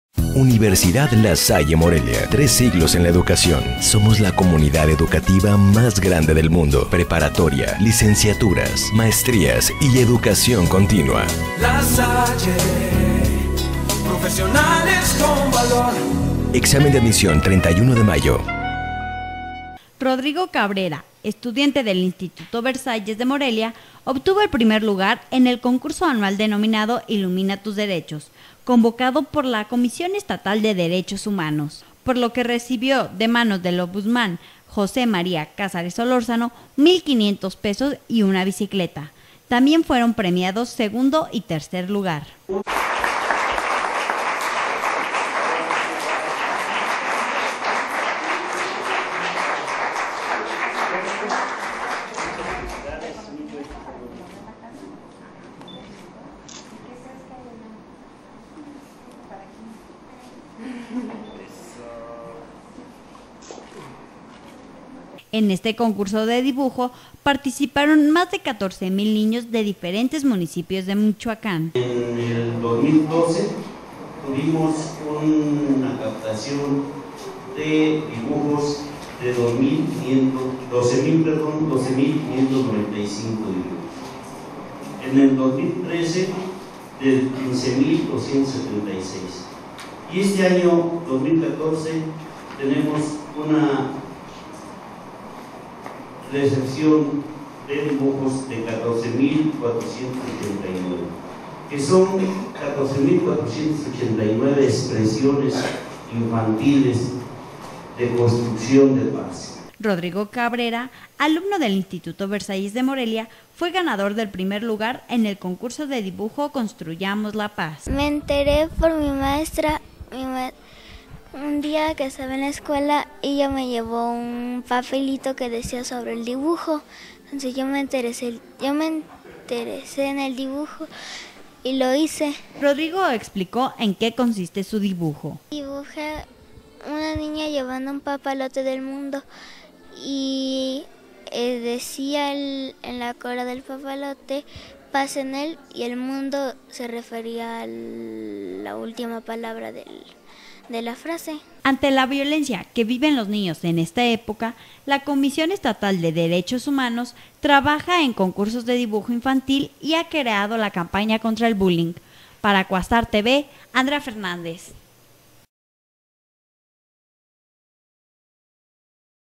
Premian dibujos ganadores de ''Ilumina tus derecho''
Rodrigo Cabrera, estudiante del Instituto Versalles de Morelia, obtuvo el primer lugar en el concurso anual denominado "Ilumina tus derechos", convocado por la Comisión Estatal de Derechos Humanos (CEDH), por lo que recibió del ombusman, José María Cázares Solórzano, mil 500 pesos y una bicicleta. También fueron premiados segundo y tercer lugar...